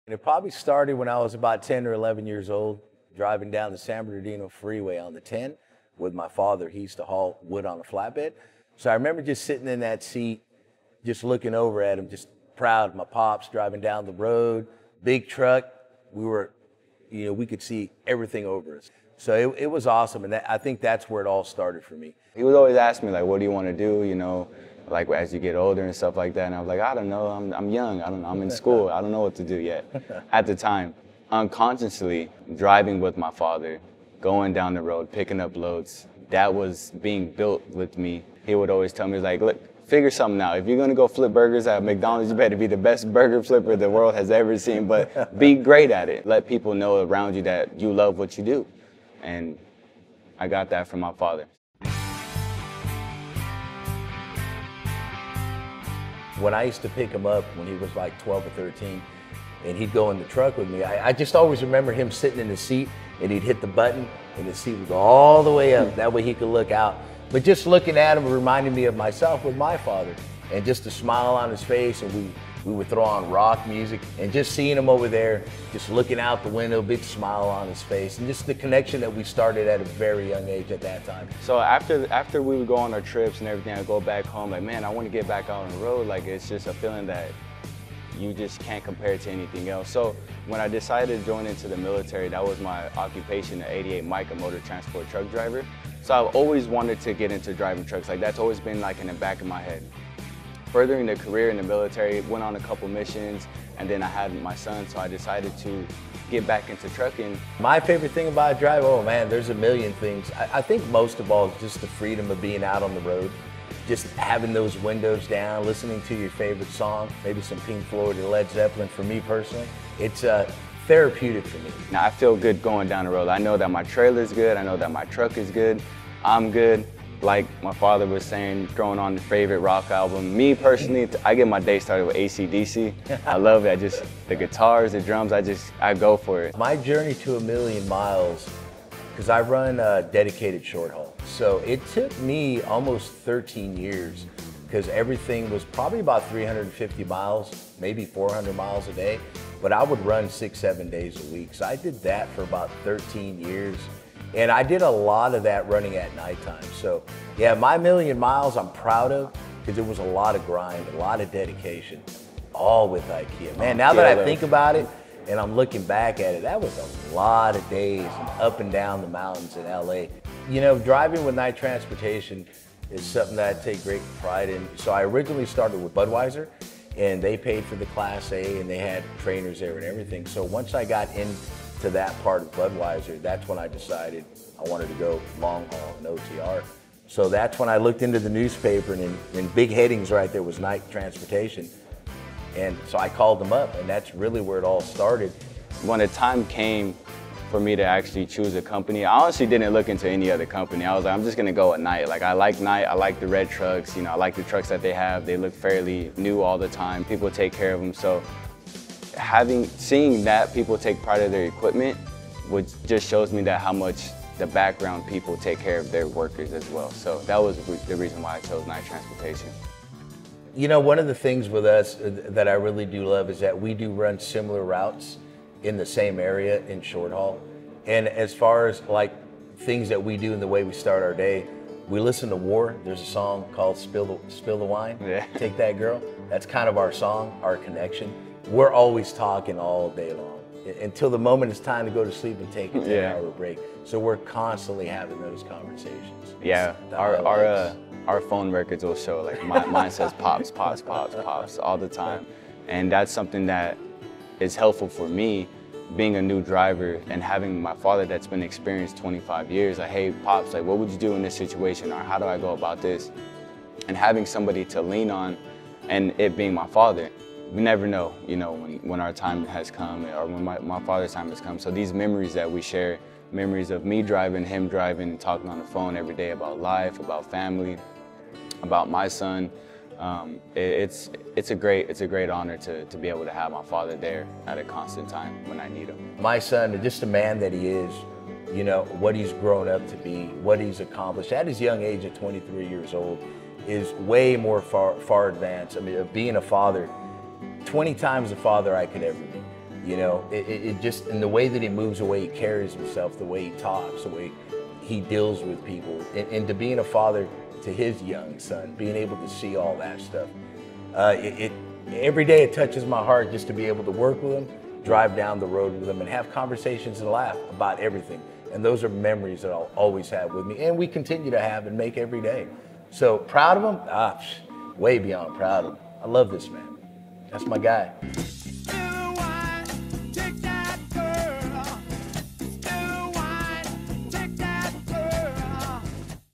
Tony & Bryson | Dialogue with a Driver
Tony and his son Bryson have a great relationship, fostered through years of experiences together over the road. As a young kid, Tony used to take Bryson along for his hauls, which he loved even though he could only barely see over the dash at first. Years later, Tony and Bryson are now both driving their own rigs and keeping their bond strong, only now it's happening from their own driver's seats and their over the road conversations now happen over CB.